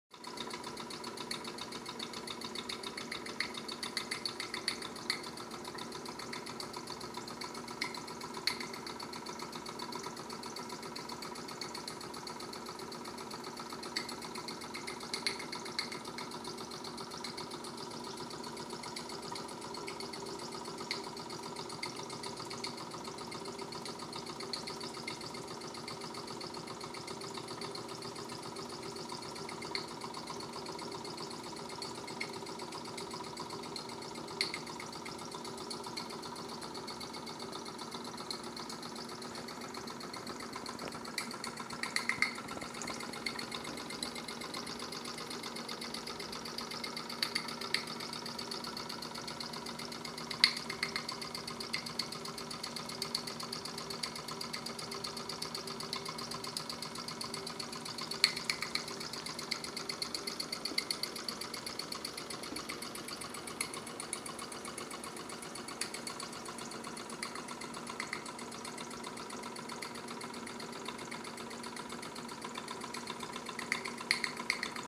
2nd run for my mamod
What a great little engine. It ran for 35 min